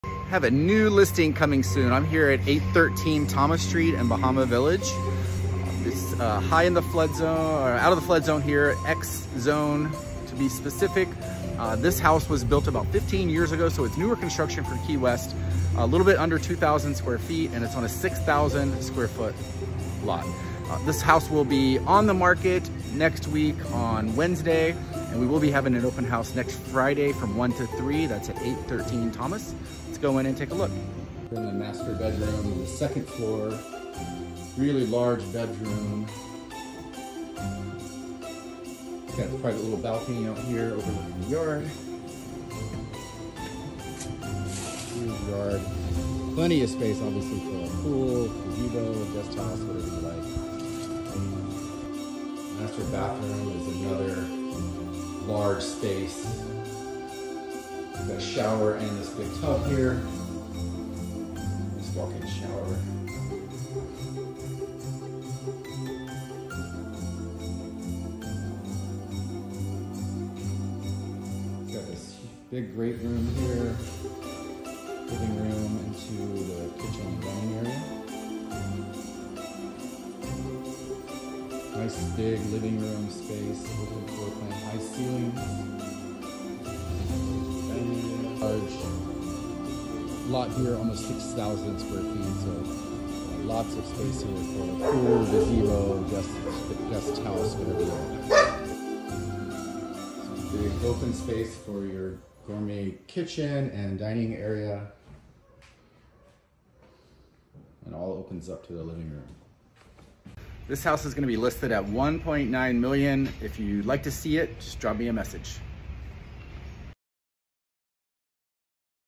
Coming Soon - 813 Thomas Street
🏡🌴 Embrace Island Living in Old Town Key West! Coming Soon! 🌊☀️

Nestled in the heart of Old Town Key West, this charming single-story home with a picturesque white picket fence awaits! Built in 2002, this 3-bed, 2-bath sanctuary spans 1865 sq ft on a generous 5856 sq ft lot.

Step inside to discover hardwood floors and a cozy fireplace, creating an inviting ambiance. The well-lit kitchen boasts granite countertops and stainless steel appliances, perfect for culinary adventures!

Located in the coveted X flood zone, this gem offers peace of mind. Plus, abundant off-street parking and ample space for a pool make this property a dream come true.

Join us for an Open House on Friday, 12/29, from 1-3 pm, and explore this paradise listed at $1.9M! Don't miss the chance to make this your Key West oasis!